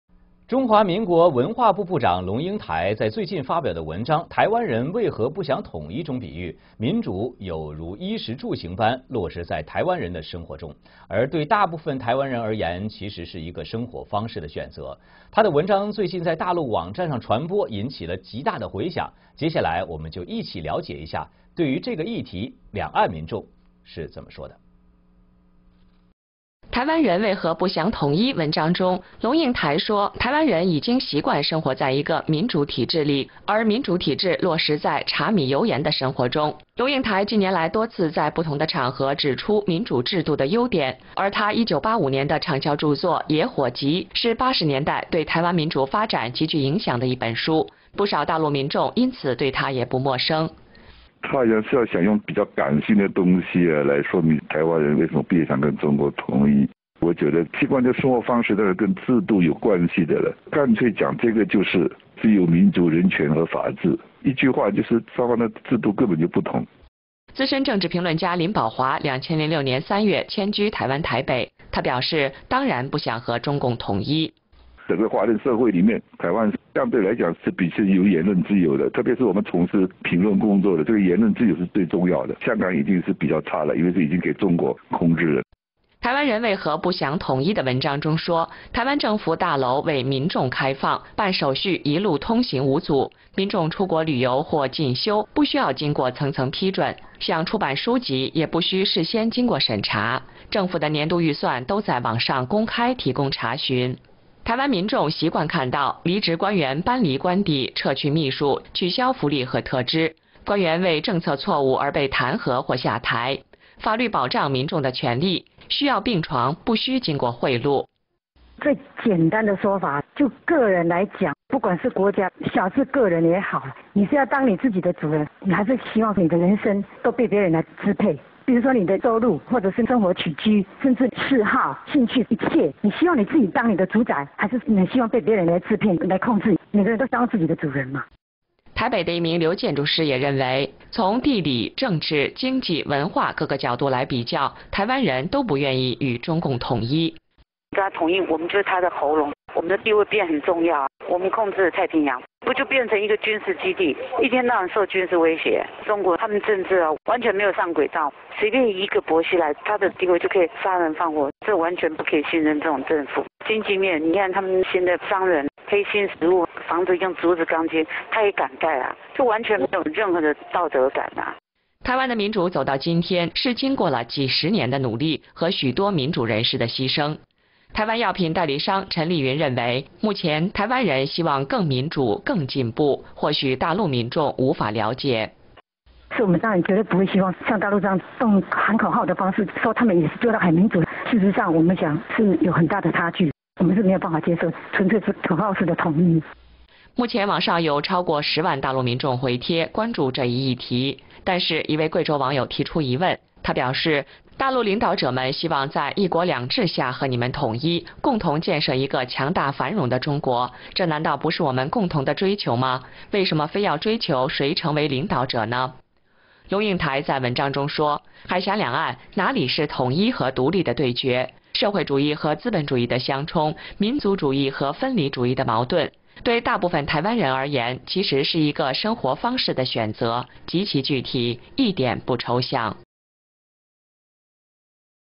Why Taiwanese People Don't Want "Unity"
Long Yingtai, head of Taiwan Ministry of Culture published a
recent article "Why Taiwanese do not want Unity".
It said democracy is like food, clothes and homes,
it is implemented in Taiwanese people's lives.
Yet "for most Taiwanese, it is a lifestyle choice." Her article
has incited many responses on Mainland websites.
Now let's take hear what Taiwanese
and Chinese have to say about it.

Long says in the article that Taiwanese are used to
living in a democratic system,
which is implemented through practicality of everyday life.

This year, Long has pointed out the advantages of a
democratic system in many different instances.
"The Wild Fire", her best selling work in 1985, had a great
impact upon Taiwan's democratic development in the '80s.
Many Mainlanders became familiar with her through
this book.

Veteran political commentator Lin Baohua: "She wants to
express sensibly why Taiwanese do not want to unify with China.
I feel habits and lifestyles definitely have something
to do with the [political] system.
Simply, that is freedom, democracy, human rights and
ruled by law.
In one sentence, the two have fundamentally
different systems."

Lin Baohua expresses, 'Of Course'
Taiwan does not want to unify with the CCP.

Lin: "In all of Asian society, relatively speaking, Taiwan has
freedom of speech, especially us who work in commentating,
freedom of speech is the most important. Hong Kong is
already worse because it has been controlled by China."

In "Why Taiwanese do not want Unity", it said Taiwan
government buildings are open for the public;
completing procedures are smooth; citizens do not need
layers and layers of permission to travel out of the country or advance studies,
no censorship is required before publishing books,
government's annual budget is posted openly online.

Taiwanese are used to seeing retired officials move out of
official residence, remove their secretary, cancel welfare and special compensation.
Officials step down for making mistakes on policies.

Laws protect people's rights.
One does not need to bribe to get a hospital bed.

Chen Liyun, Taiwan pharmaceutical representative said:
"The easiest way to put it is: do you want to be your own boss or hope your life is dominated by others?
For example your income or daily life, even in your hobbies and
interests, you hope to be your own boss, or hope someone else controls it, can you then be your own boss?"

Architect Liu from Taipei also believes, to compare from all
angles; geographically, politically, economically and culturally,
that Taiwanese do not want to unify with the Chinese
Communist Party (CCP).

Architect Liu: "We are their state-run media if we unify with
them. Our position is very important, we control the Pacific.
But did it not become a military base, being threatened day
and night?
China's politics are completely not on track, a position like
Bo Xilai's allows him to do whatever bad things he wants.
This type of government is completely not trustworthy.

Economically, look at their businessmen, black hearted fools,
they even dare to use bamboo as steel bars for houses.
There is completely no sense of morality."

The democracy in Taiwan today came from dozens of years
of efforts and the sacrifices of many democratic activists.
Chen Liyun, Taiwan pharmaceutical representative, believes
Taiwanese hope for more democracy and more advancement right now.
Maybe it is something that Mainlanders are unable
to understand.

Chen Liyun: "So, of course we absolutely do not hope to
be like Mainland and say that we are also very democratic.
In reality we are very different. We cannot accept the
tradition of empty slogans."

Currently, more than 100,000 Mainlanders responded to
this topic online. However, a netizen from Guizhou is skeptical.
He said: "Mainland leaders hope to unify with you under 'One
Country, Two Systems' rule and build a strong and prosperous
China together, is this not our common pursuit?
Why insist on pursuing who becomes the leader?"

Long Yingtai says in the article: "Where is the showdown
between Taiwan and China for unity?
The clash between socialism and capitalism?
The conflict between democracy and separatism?
For most Taiwanese, it's a matter of lifestyle choice,
very specific, not abstract at all."